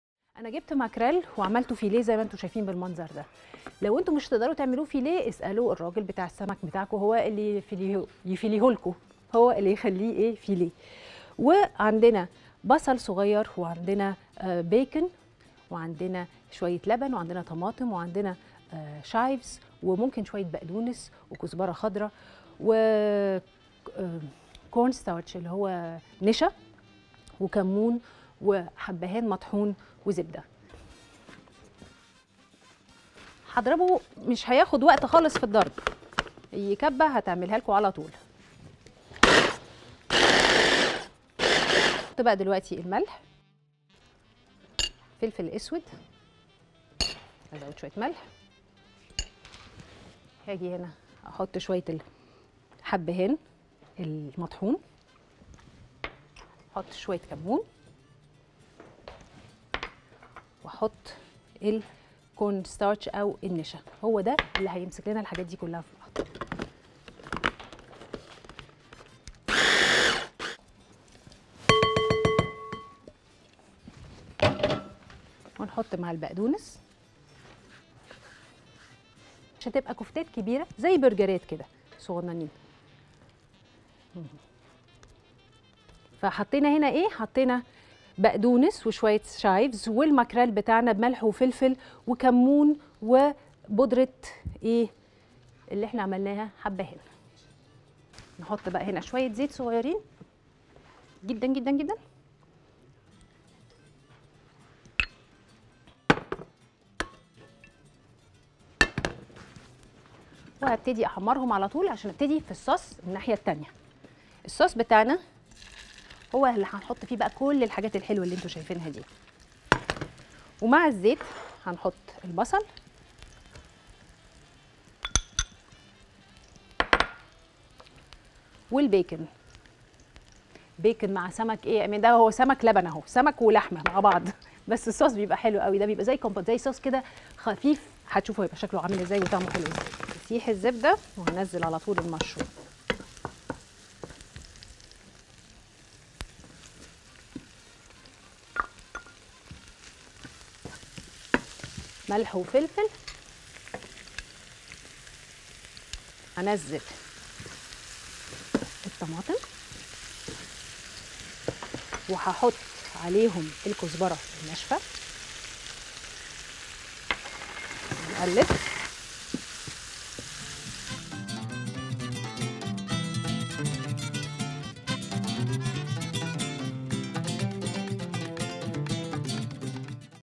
كفتة ماكريل | أماني رفعت
طريقة عمل كفتة ماكريل مع أماني رفعت في بالهنا